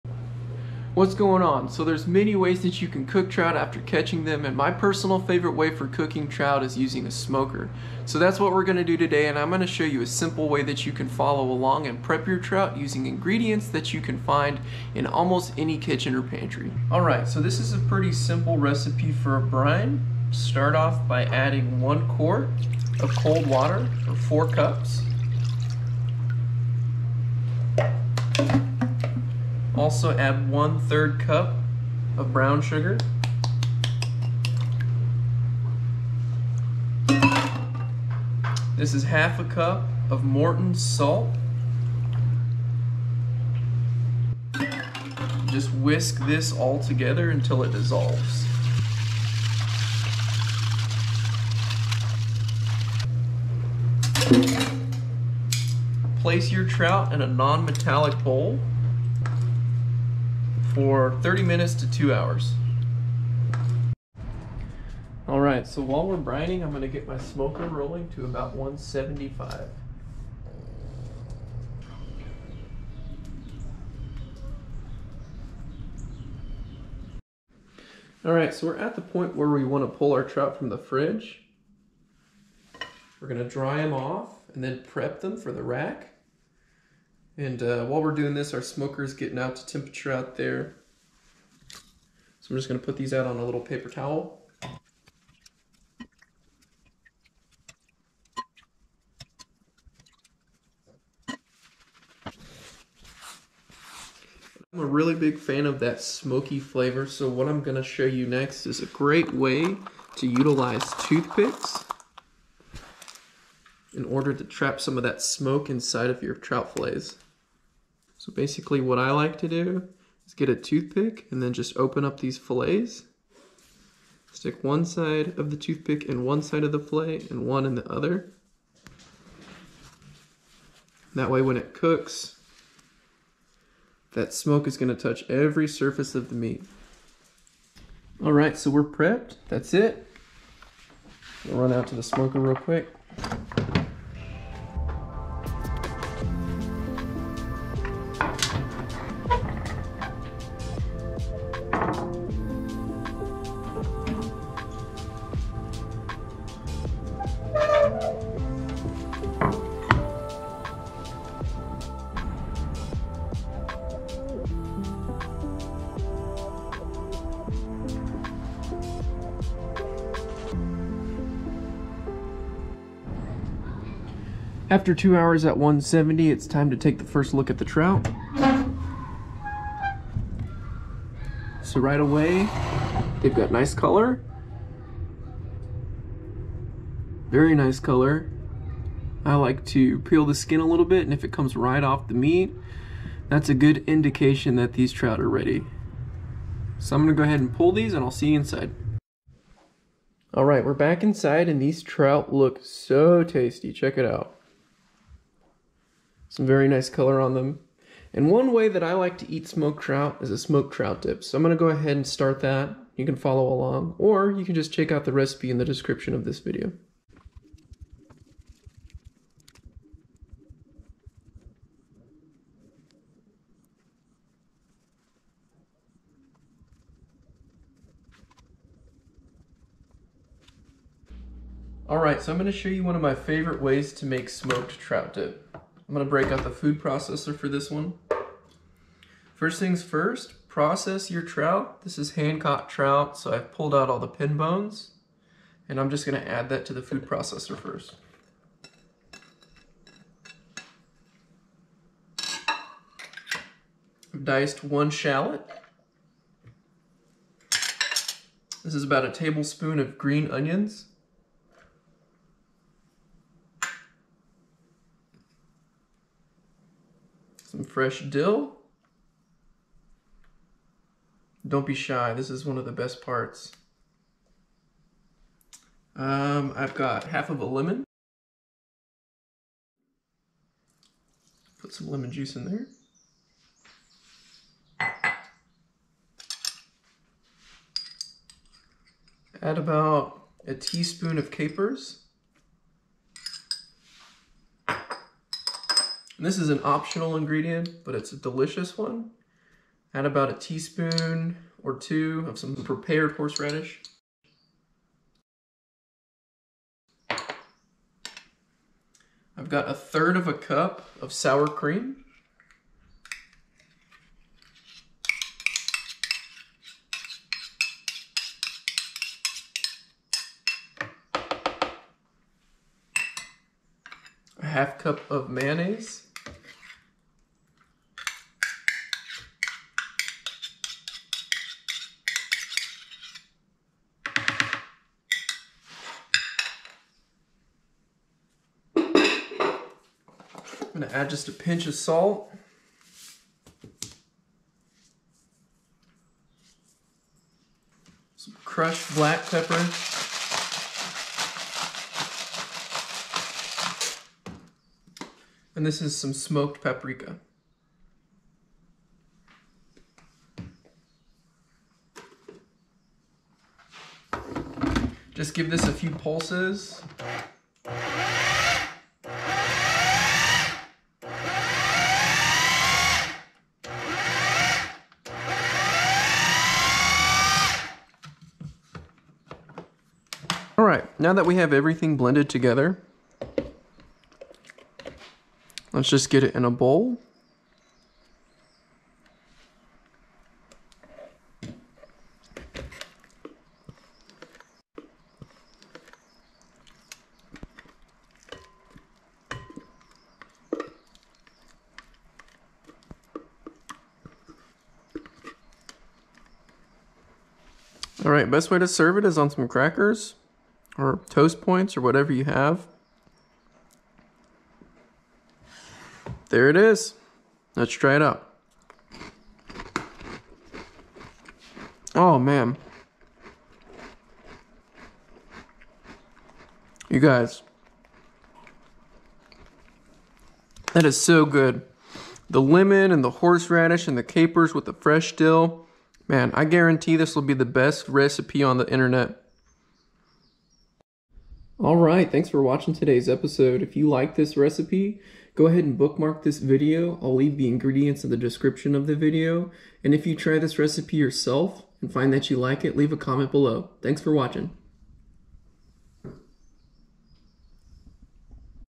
SMOKED TROUT DIP | Brine, Smoke, & Dip | Catch & Cook
CurlyTailFishing's Smoked Trout Dip Recipe
◦ 12oz trout, skin removed, flaked
◦ 1/2 cup fresh dill
◦ 1 tbsp green onion, diced
◦ 1 shallot, diced
◦ 1/2 lemon, juiced
◦ 1 tsp capers
◦ 1 tsp prepared horseradish (optional)

Step 1 - Brine
Combine 1 quart of cold water (4 cups), 1/2 cup salt, 1/3 cup of brown sugar. Cover and refrigerate for 30 minutes to 2 hours.

Step 2 - Smoke
Place trout on cooking rack, place toothpick in fillets to hold open during the smoking process, smoke trout for 2 hours at 170-200 degrees.

Step 3 - Prepare Trout
Remove skin from trout and process out the hair bones from fillets.

Step 4 - Combine Ingredients
Combine trout, sour cream, mayonnaise, green onion, shallot, salt, crushed black pepper, smoked paprika, lemon juice, prepared horseradish in a food processor and pulse. Add to bowl and stir until blended. Cover and refrigerate for 30 minutes. Garnish with paprika, lemon slice, and freshly chopped dill.